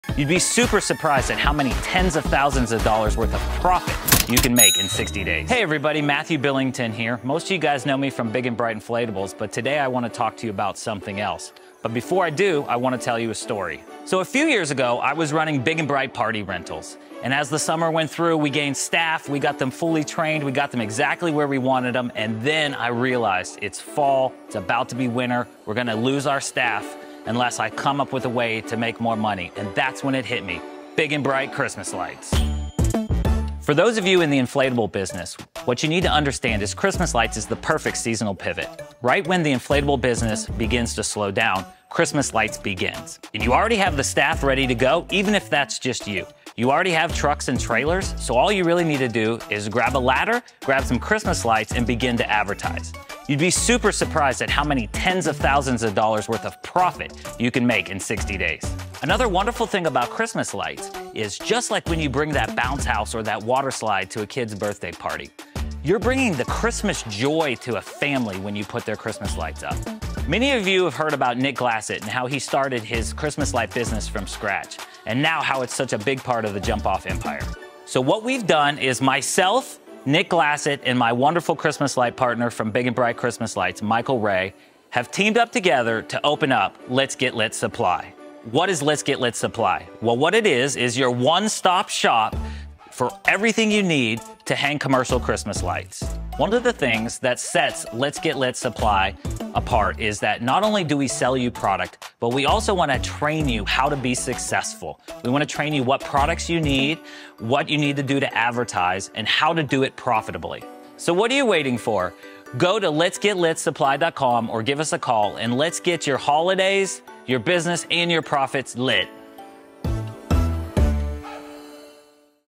Lets Get Lit Supply - Commercial Christmas Light Installer supplier
Installing Christmas Lights is a great way to make a lot of extra income in a short amount of time around the holiday season. Lets Get Lit Supply has high quality christmas light installer supplies such as a variety of Minleon C9 Lights, socket cord, clips, male and female plugs, ridge clips, mini lights and more!